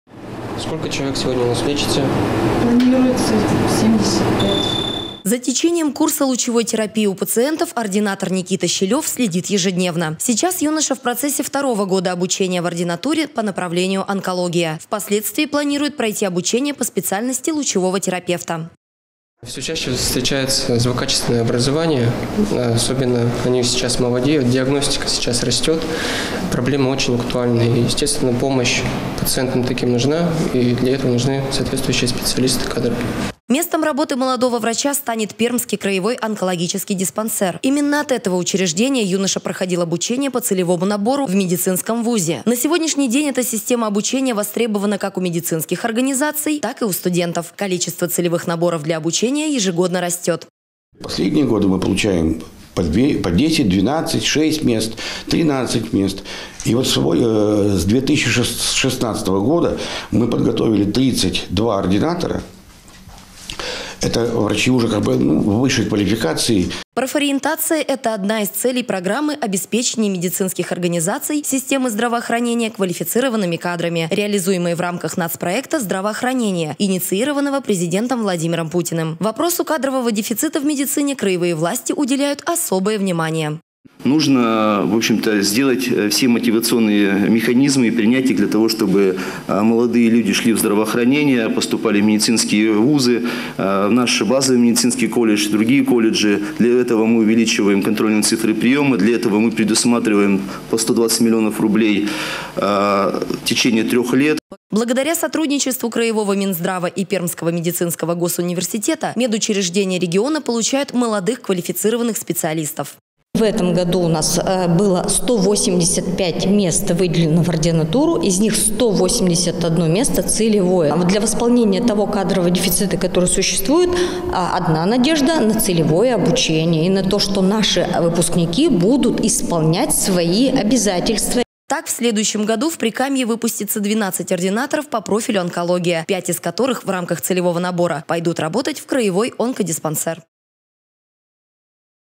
Новости Пермского края: целевое обучение_23.11.2021_СольТВ
О целевом обучении будущих врачей  сюжет наших коллег из Перми.